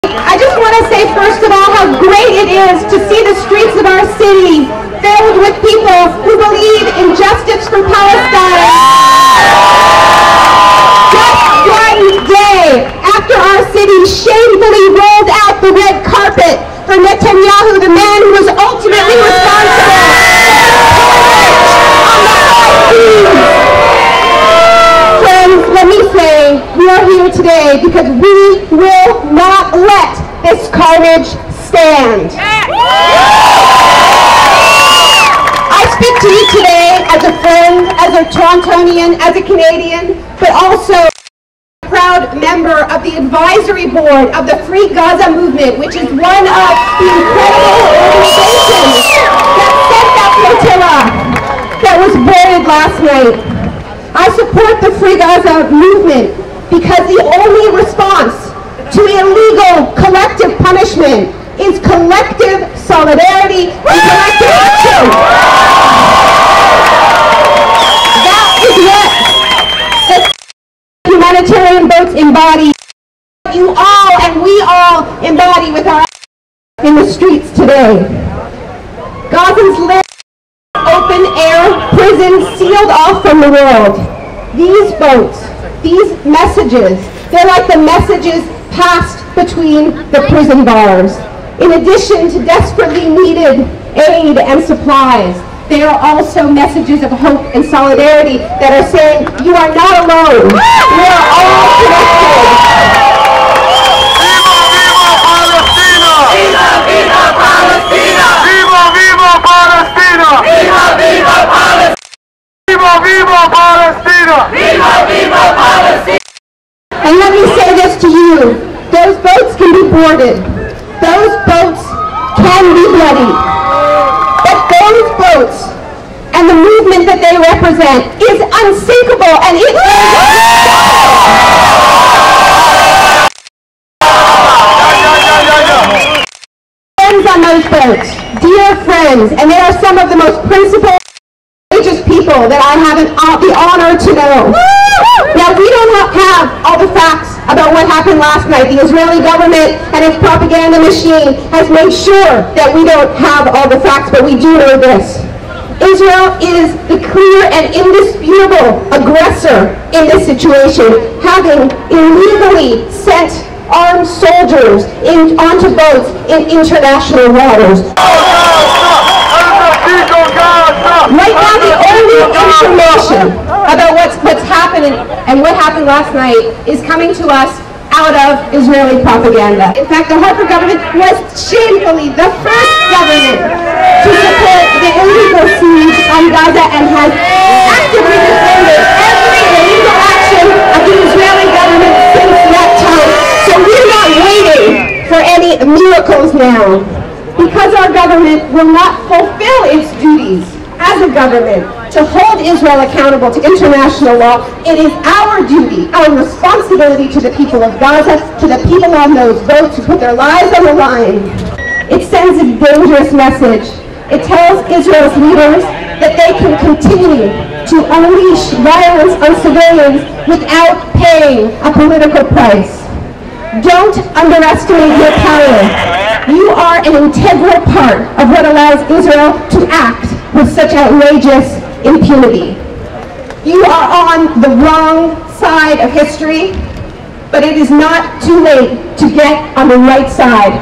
Naomi Klein speech at pro-Palestine rally
Naomi Klein (author of "No Logo"), gives a speech at the end of the pro-Palestine march at Yonge-Dundas Square on May 31st, 2010.

The march was in response to the taking of humanitarian vessels by armed Israeli forces on the same day. Organizers estimated 1,500 people attended the rally in Toronto, one of 12 in Canada.

Unfortunately, I kinda butchered this video using a variety of amateur techniques, but at least most of what she said is there. I'll post lots of photos on TorontoCityLife.com to make up for it, I promise!